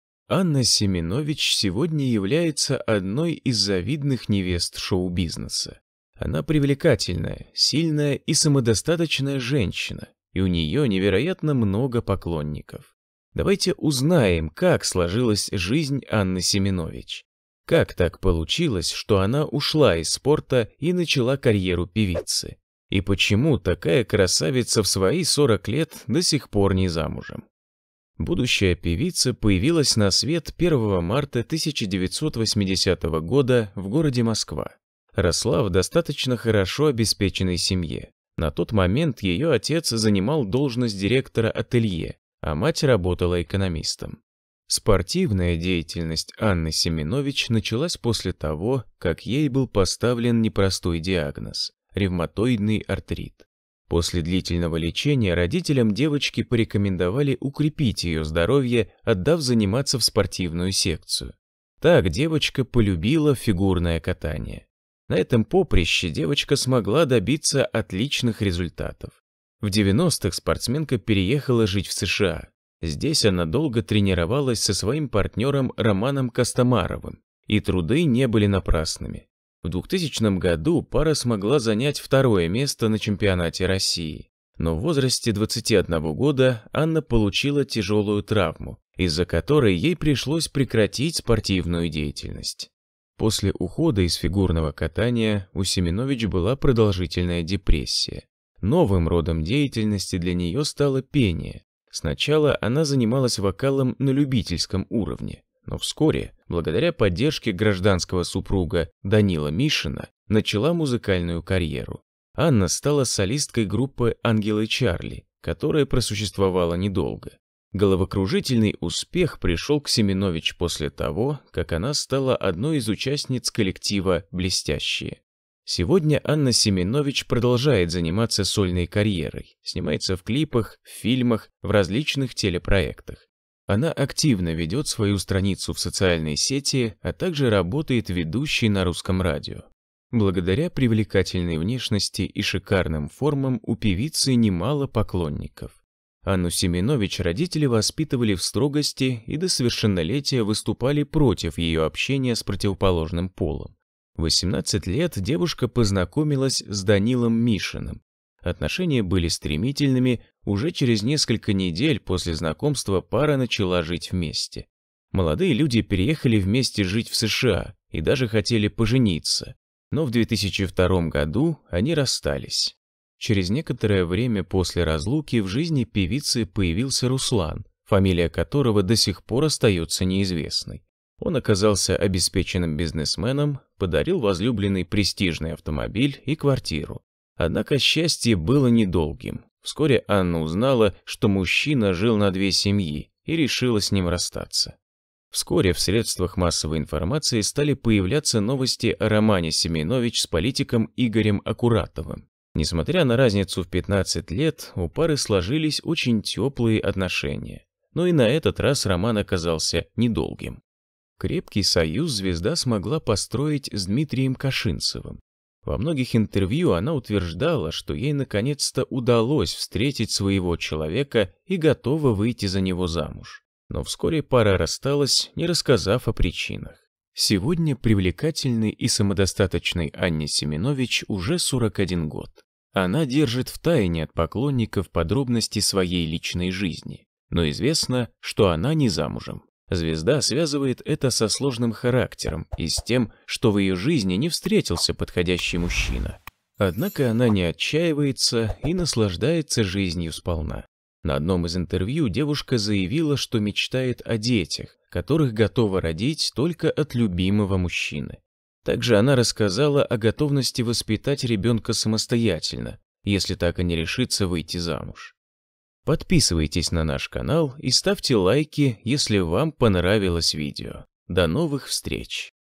КАК ЖИВЕТ АННА СЕМЕНОВИЧ? ПОЧЕМУ в 41 год певица до сих пор НЕ ЗАМУЖЕМ?
Анна Семенович сегодня является одной из завидных невест шоу-бизнеса. Она привлекательная, сильная и самодостаточная женщина и у нее невероятно много поклонников.

Давайте узнаем, как сложилась жизнь Анны Семенович. Как так получилось, что она ушла из спорта и начала карьеру певицы? И почему такая красавица в свои 40 лет, до сих пор не замужем?